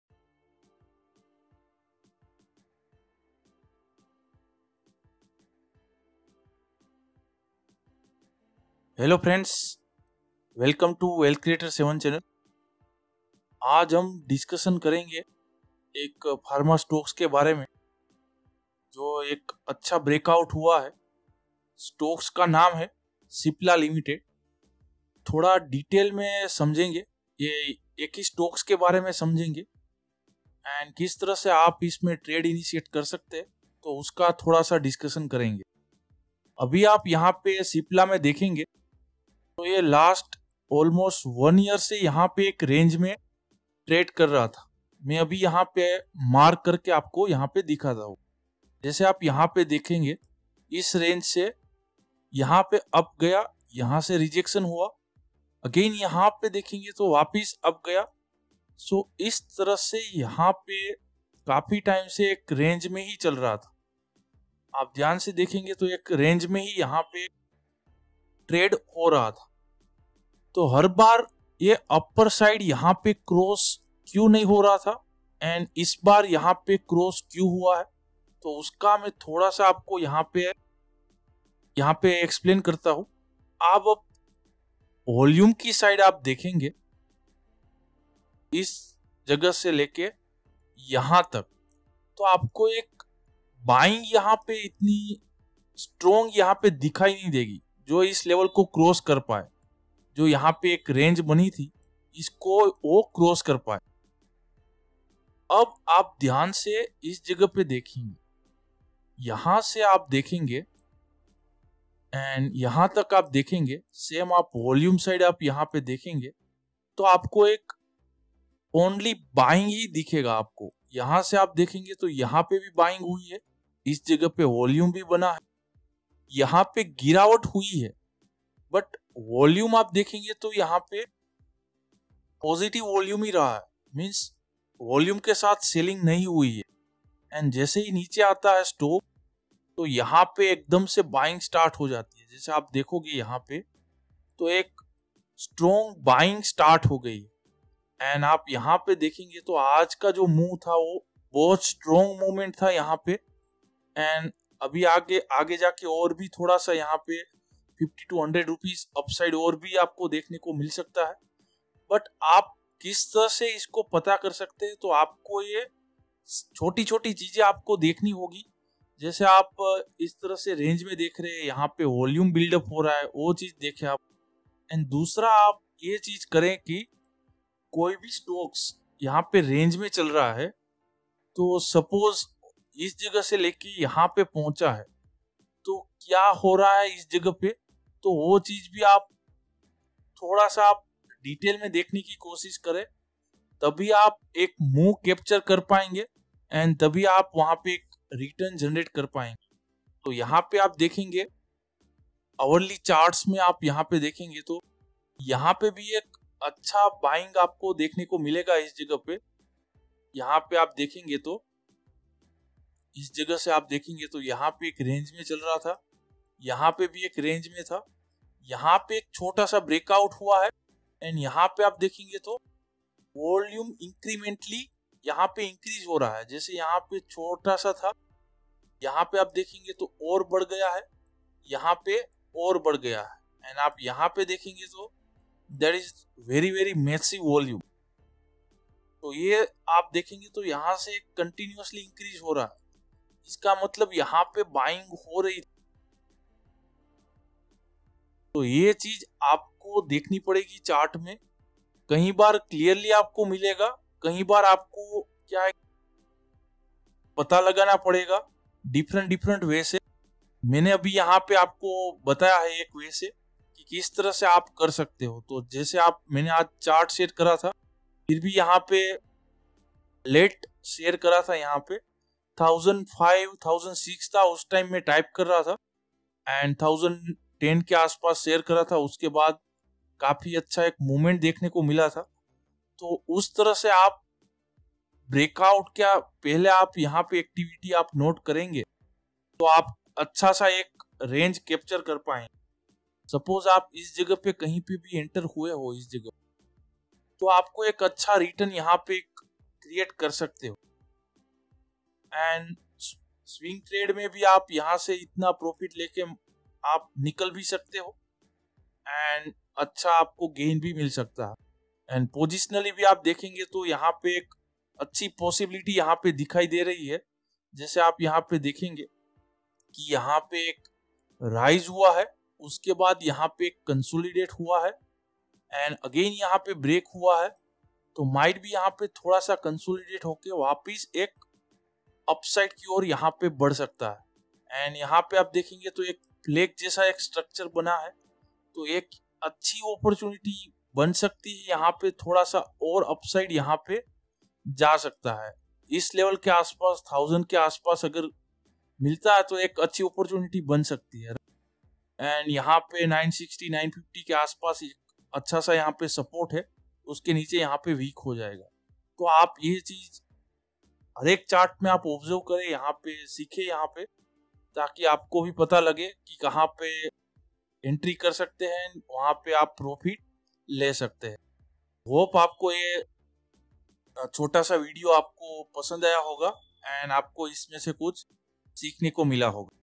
How to Enter before Breakout ? | Pharma Stock chart study | Weallthcreator7
Hi Friends in this video discuss about one pharma stock which trading in range and how it breakout and what things we need  to observe in any chart to capture movement in any stock before breakout

how to enter stock before breakout